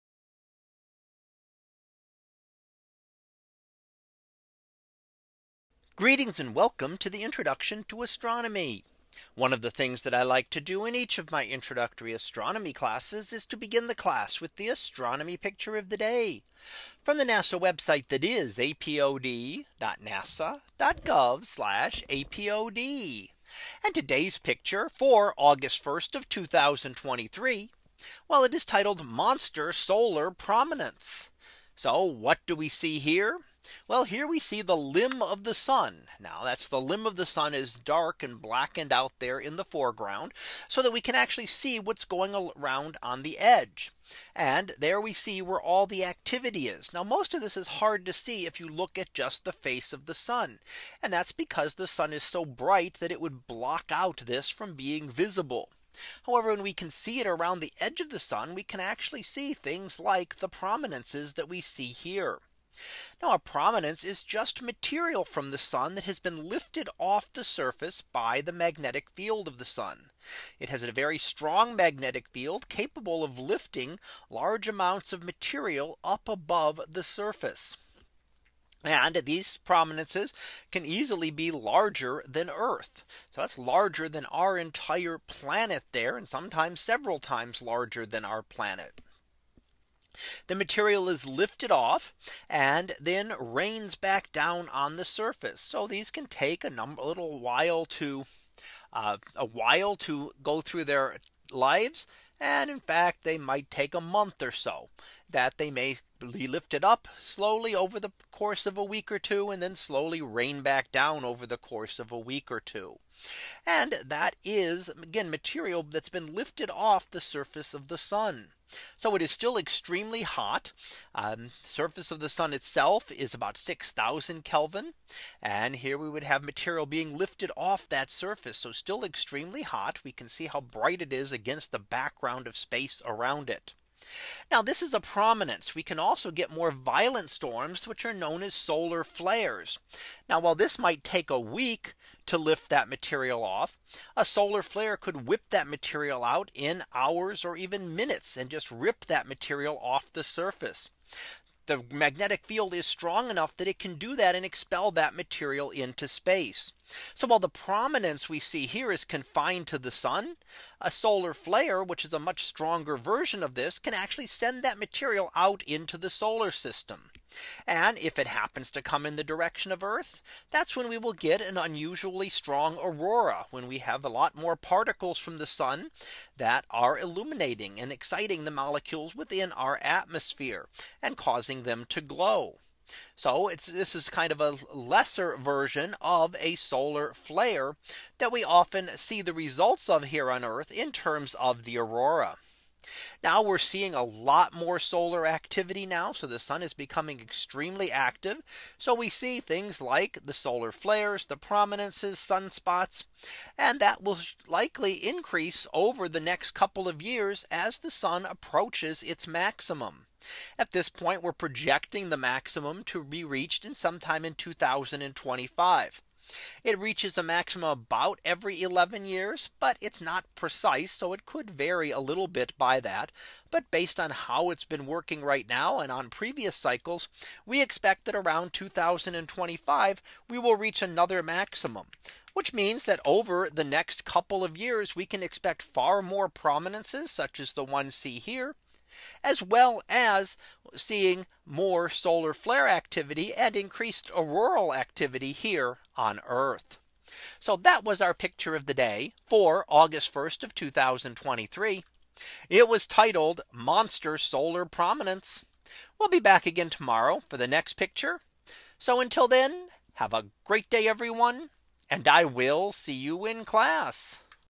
2023 August 01 - Monster Solar Prominence
In today's image, we see a giant solar prominence as seen along the limb of the Sun. These occur when material from the solar surface is lifted by the Sun's magnetic field and then rains back down on the surface. These can easily be larger than our own planet!